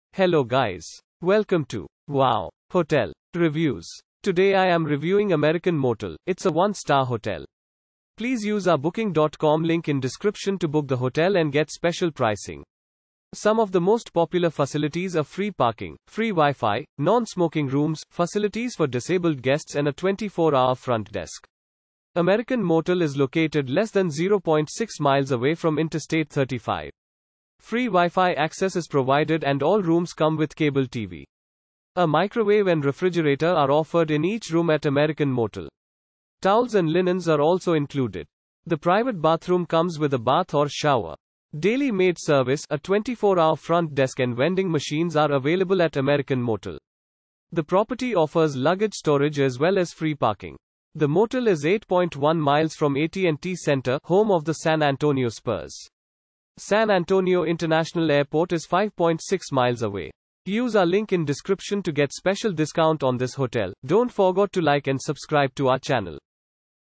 American Motel Review - San Antonio , United States of America
American Motel

American Motel is located less than 0.6 mi away from Interstate 35. Free WiFi access is provided and all rooms come with cable TV.
A microwave and refrigerator are offered in each room at American Motel. Towels and linens are also included. The private bathroom comes with a bath or shower.
Daily maid service, a 24-hour front desk and vending machines are available at American Motel. The property offers luggage storage as well as free parking.
The motel is 8.1 mi from AT&T Center, home of the San Antonio Spurs. San Antonio International Airport is 5.6 mi away.